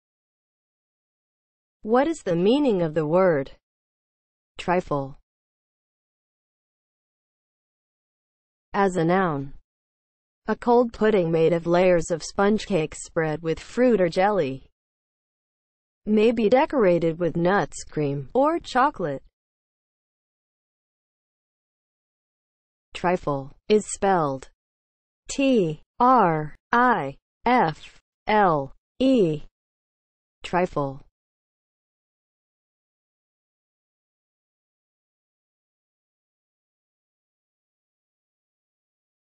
What is the meaning of the word TRIFLE?
Definition and spelling of the word TRIFLE.

(00:00) Intro
(00:07) As a noun
(00:21) Spelling

As a noun (00:07):
• a cold pudding made of layers of sponge cake spread with fruit or jelly; may be decorated
  with nuts, cream, or chocolate